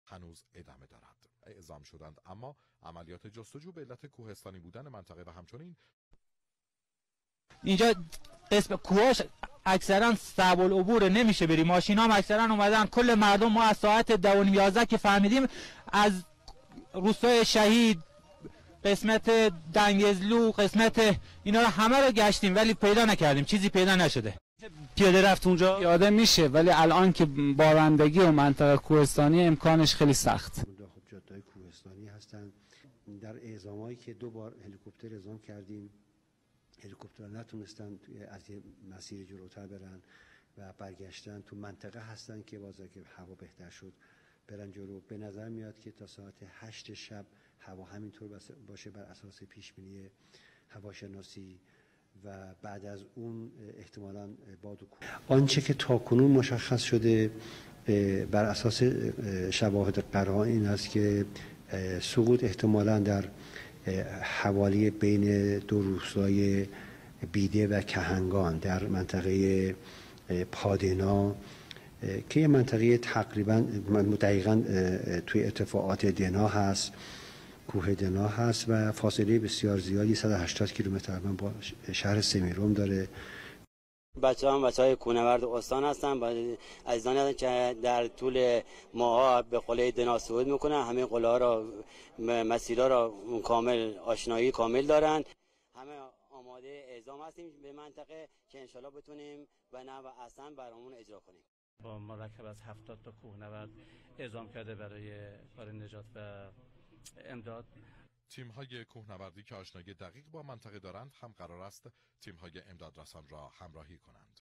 Ҳавои бад ҷустуҷӯи ҳавопаймои сарнагуншудаи эрониро душвор кард.
Ҳавои бад ҷустуҷӯи ҳавопаймои сарнагуншудаи эрониро душвор кард. Ба қавои мақомоти эронӣ дар ин ҳодиса 66 кас ҳалок шудаанд.